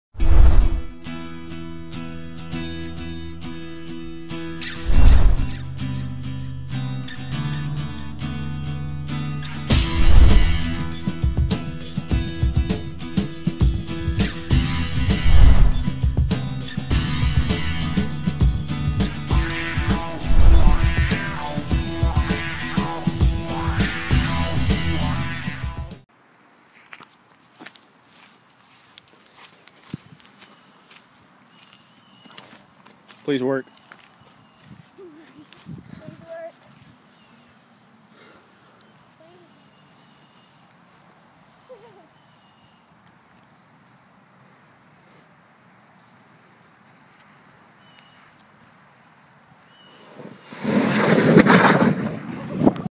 Sparkler Bomb Fail
Levi lights a 250 sparkler bomb that spirals out of control and almost hits Matt!